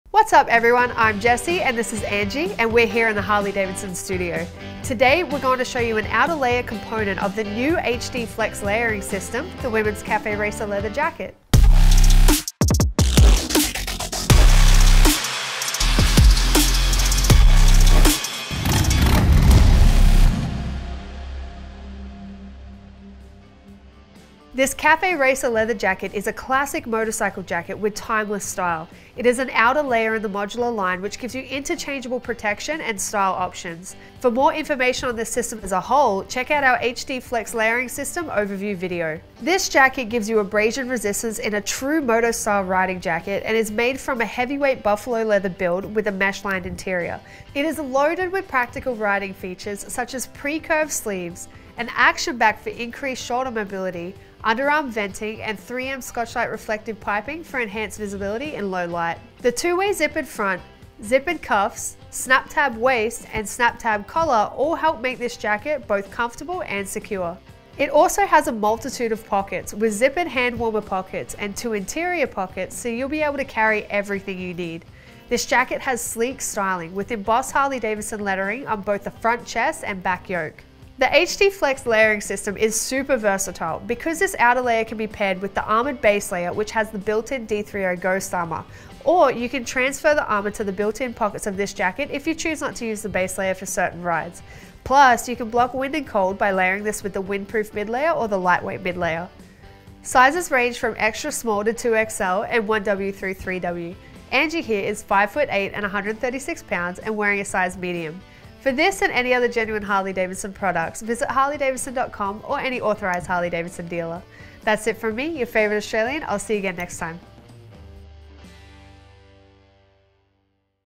Harley-Davidson Women’s Flex Layering System Café Racer Leather Jacket Overview
Our Harley-Davidson Flex Layering System features quality functional materials for specific purposes at each level to keep you warm, mobile, and protected for endless riding. Working together they formulate our most protective riding solution.

For abrasion resistance that doesn’t cut corners on true moto style, check out our Flex Layering System Café Racer Leather Jacket. This is layer three in our 1-2-3 modular line that gives you interchangeable protection and style options for riding.

Its heavyweight buffalo leather build is loaded with practical riding features like an action back, pre-curved sleeves, underarm venting, and reflective piping for added visibility. Its sleek styling looks great as a stand-alone with our embossed namesake on the front chest and back yoke.

Get impact protection by combining this layer with our Flex Layering System Armored Base Layer/ Then block wind and cold by adding our Flex Layering System Windproof Mid Layer or our Flex Layering System Lightweight Mid Layer.